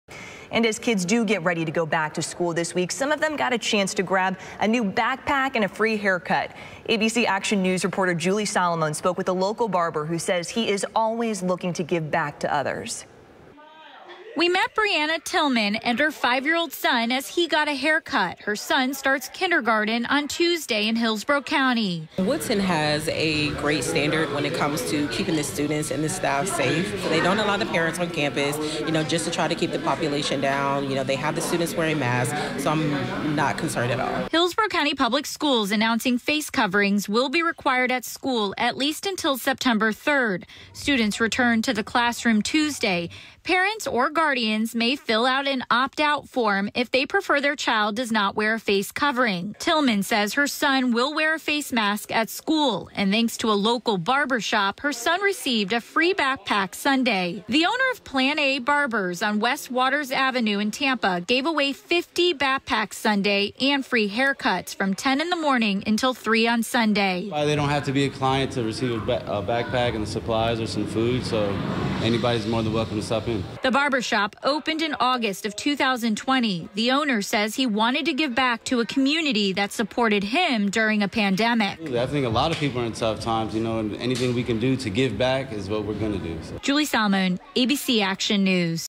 Tampa barbershop gives out free backpacks and haircuts
A barbershop in Tampa gave away free backpacks and haircuts as children head back to school this week.